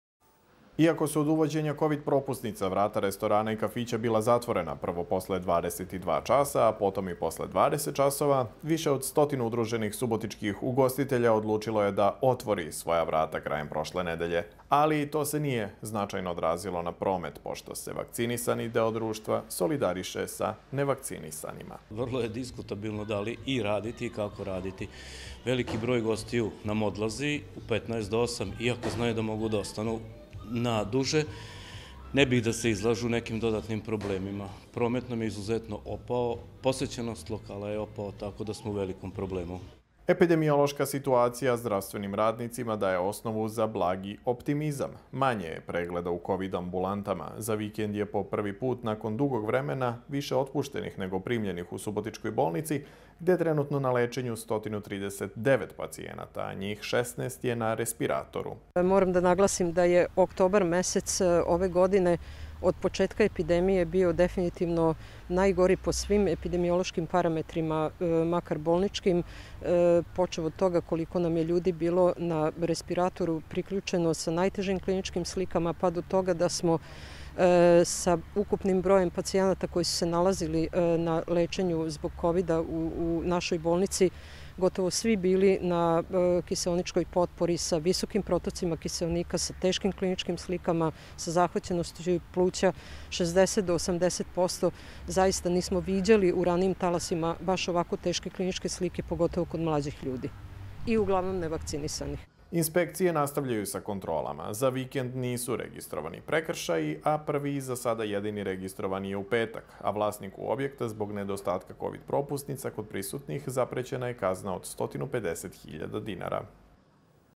Raste interes subotičkih ugostiteljskih radnika za vakcinaciju
Protiv odgovornih u jednom ugostiteljskom objektu u Subotici prošle nedelje pokrenut je prekršajni postupak sa zaprećenom kaznom od 150.000 dinara zbog nepoštovanja epidemijskih mera koje se odnose na kovid propusnice. Iskustva ugostitelja su takva da se vakcinisani solidarišu sa nevakcinisanima i napuštaju objekte u petnaest do osam. Za to vreme raste interes ugostiteljskih radnika za vakcinaciju.